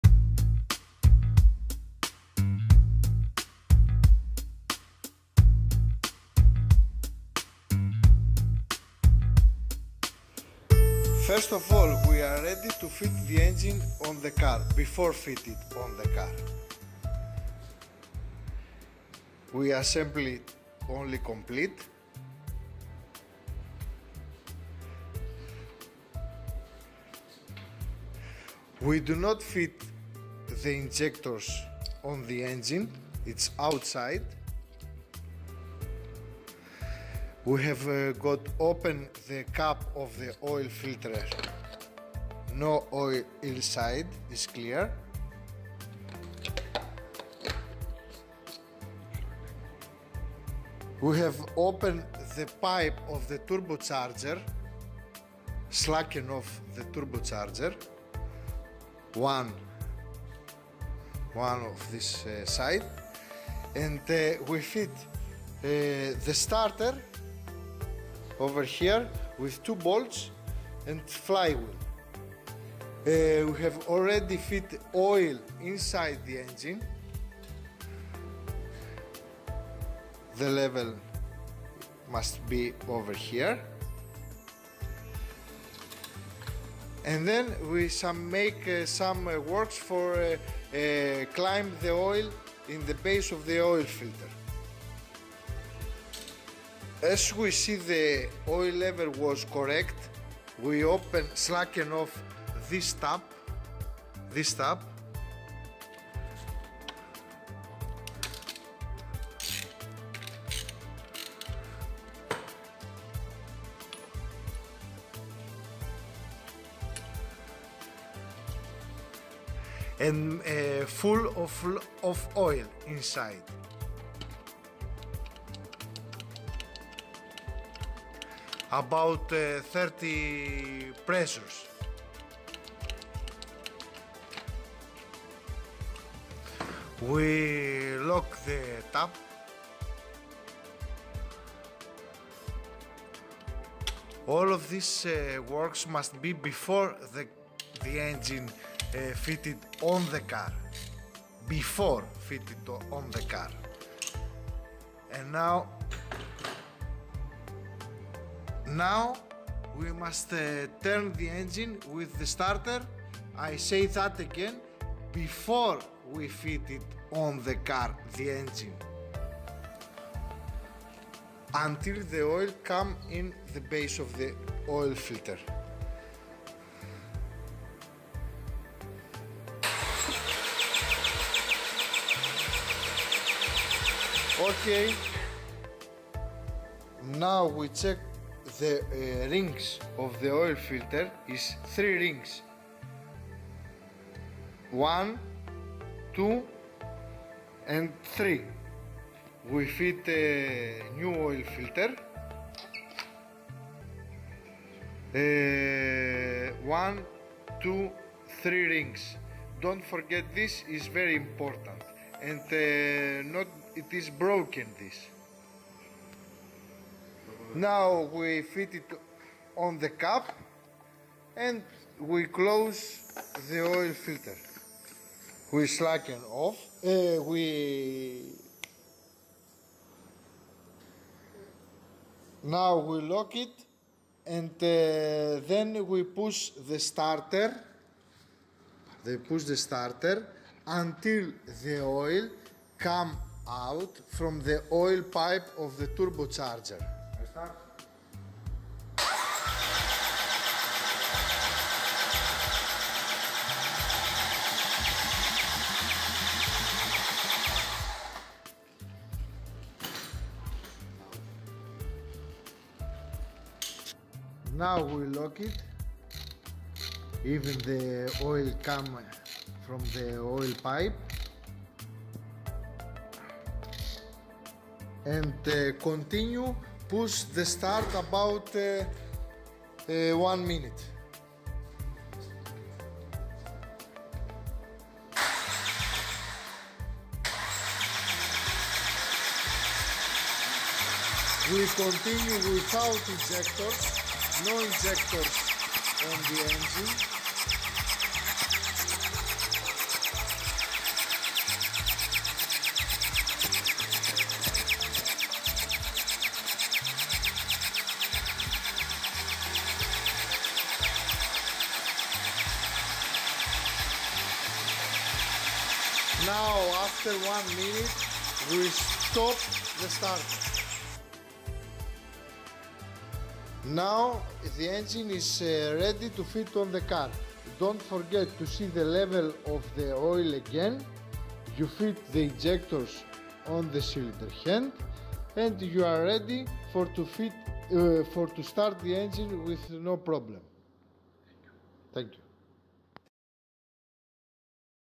Montaje OM611 OM612 OM646
Preparacion para el correcto montaje de los motores OM611 - OM612 - OM646 de Mercedes, efectuarlo antes de la instalación en la unidad para evitar daños en el motor.
Diesel Parts. Especialistas en componentes de motor